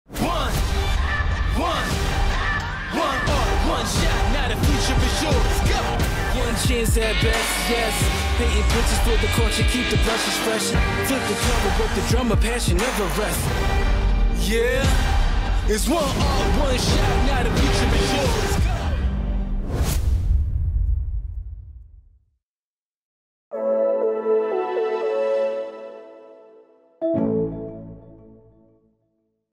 ONE Fight Night 23 생중계 예고 | 쿠팡플레이 | 쿠팡
8대 ONE 라이트급 챔피언 옥래윤
한국인 최초! ONE 챔피언십 타이틀전 3회 도전기

14연승 무패 파이터 라술로프를 누르고
다시 한번 챔피언의 벨트를 차지할 수 있을까?

'Smasher' 강지원과 크를 흐롄코의 매치까지!

진짜들의 숨 막히는 대결
오직 쿠팡플레이에서 만나보세요!

📌  중계 일정
7/6(토) 오전 9시
옥래윤 vs 라술로프 / 강지원 vs 크를 흐롄코